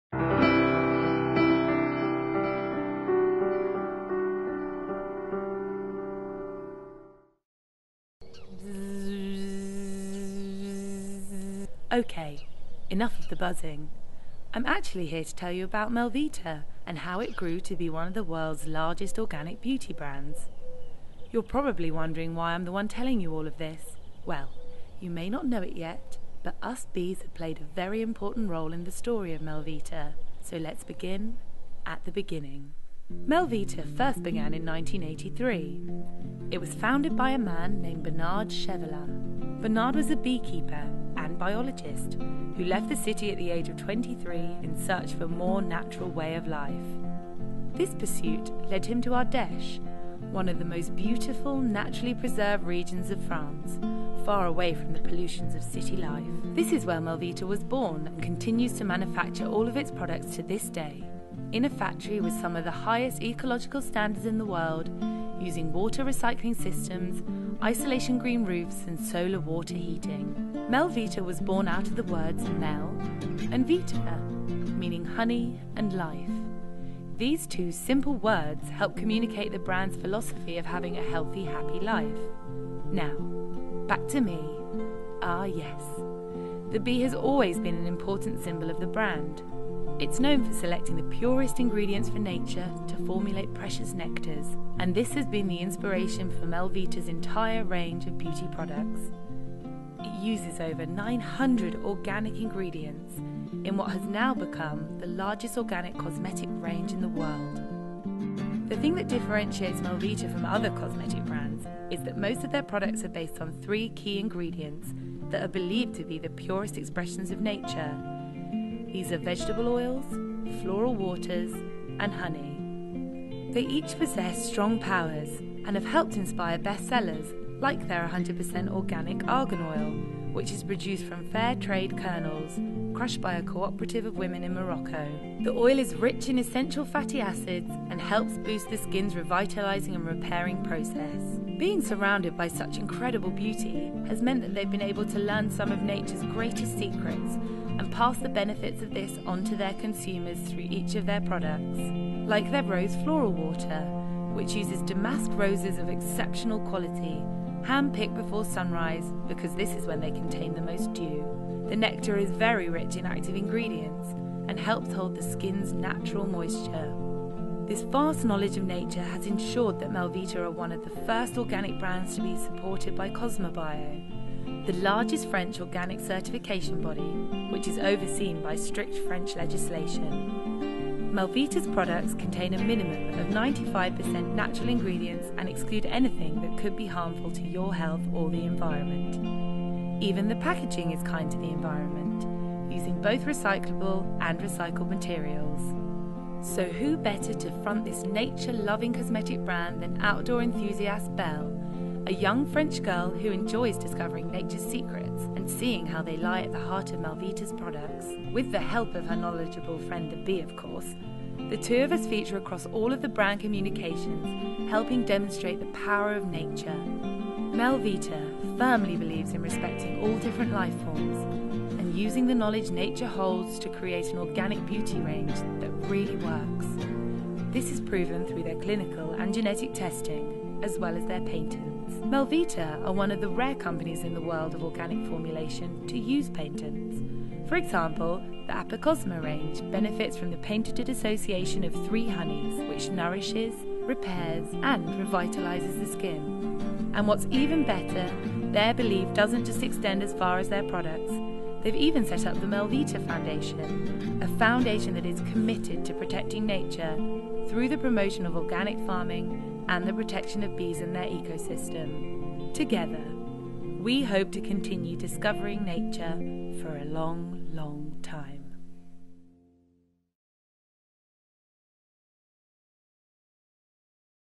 Melvita | The Brand Story
Born deep in the French countryside, Melvita is an iconic range of
organic skincare that promises to use only the best natural
ingredients, to provide you and your skin with a gentle yet effective
regime. Melvita Research & Development team, led by founder Bernard
Chevilliat, are at the forefront of organic formulation, and continue
to use innovative ingredients from all over the world to offer only
healthy, effective beauty.